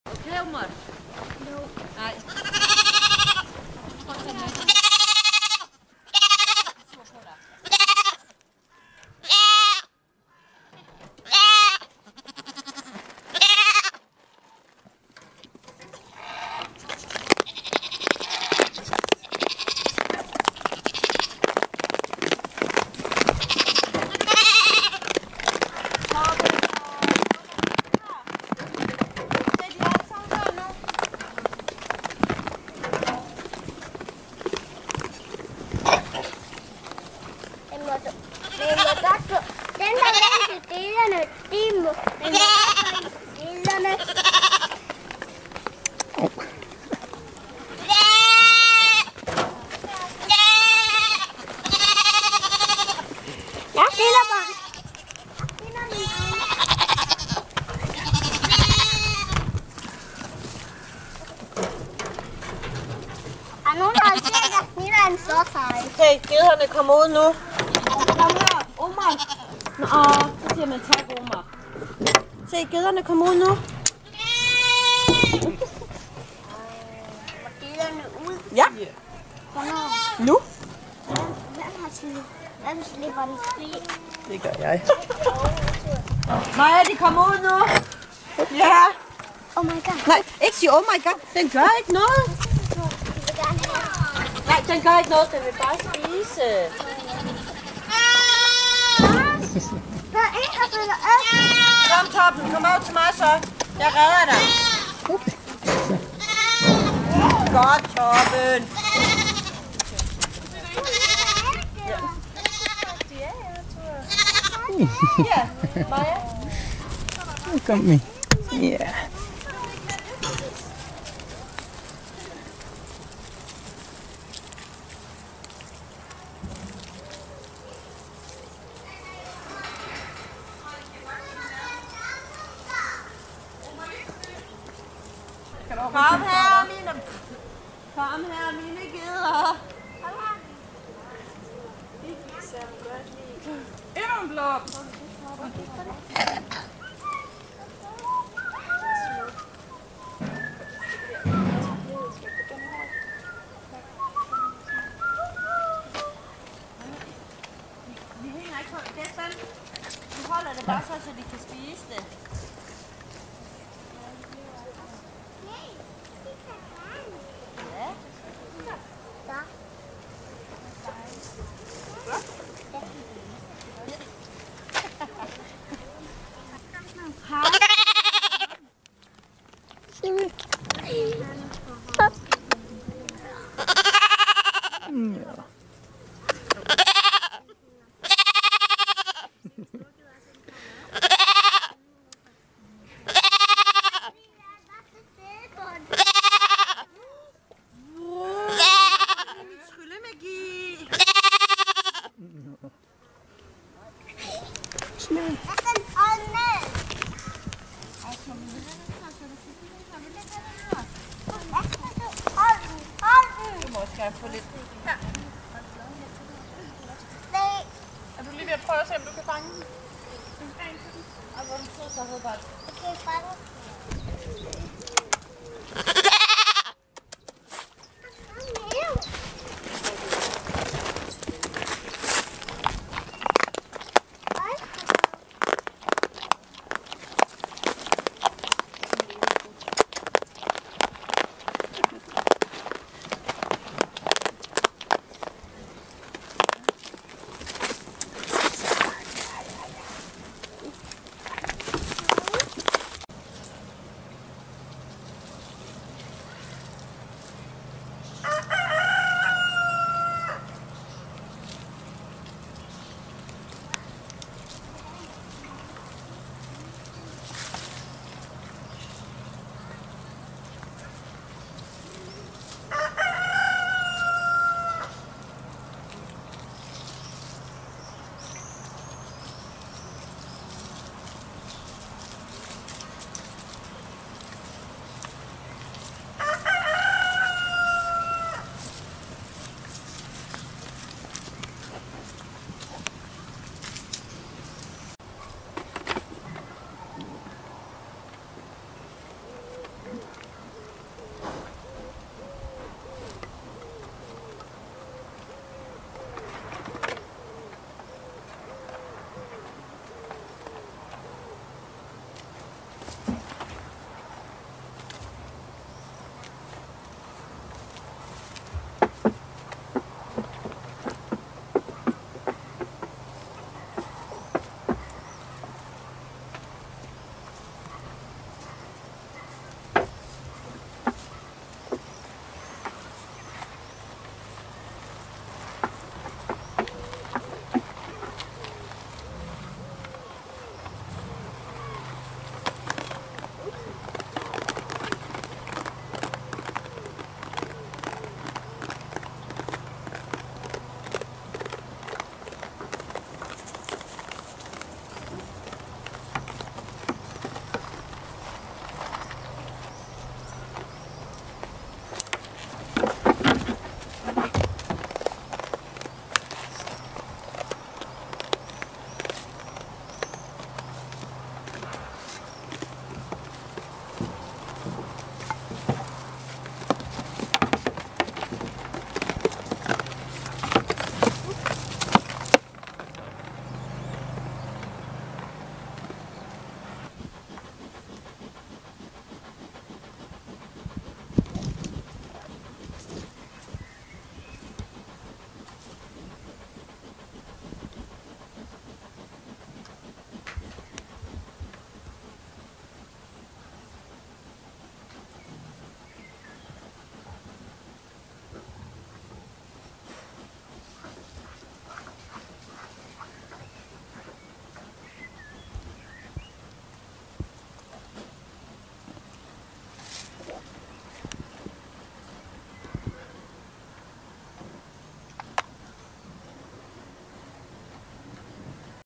goatlog: Panting goats (2019-07-26)
You can tell it's a hot day when the goats are panting in the heat.